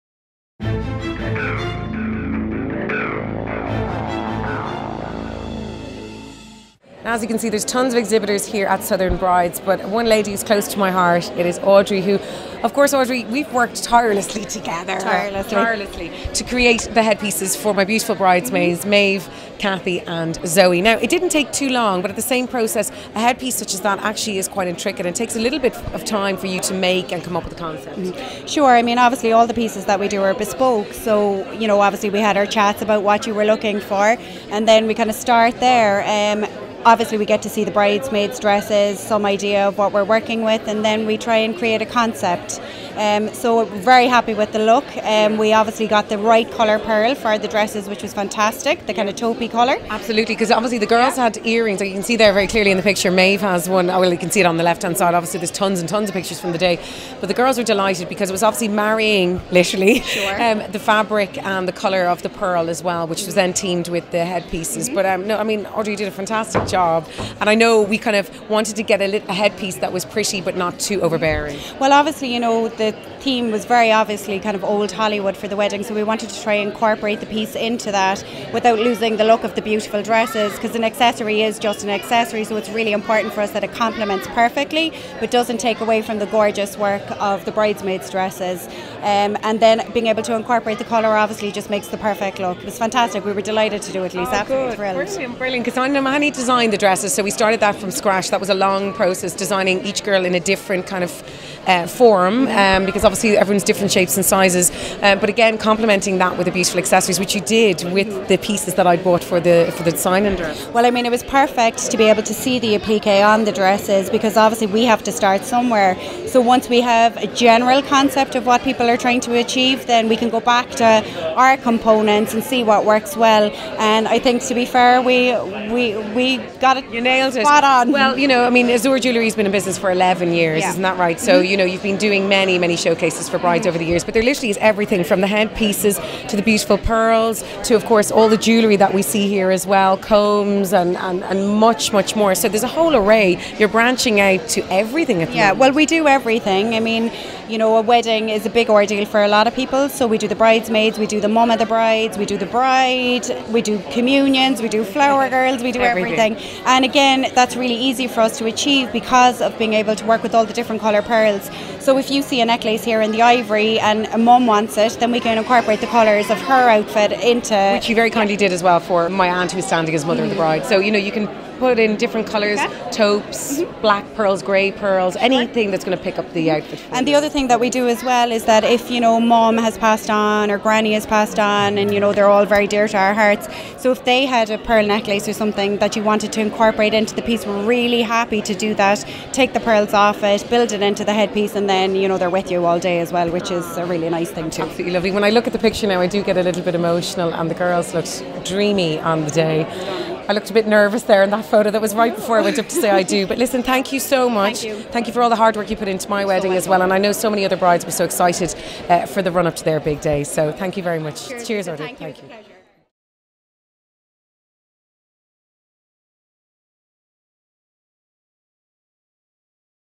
Lisa Cannon at Southern Brides Cork - Lets Talk Jewellery
Lisa Cannon and Audrey Lehane discuss the finer points of getting your wedding accessories right.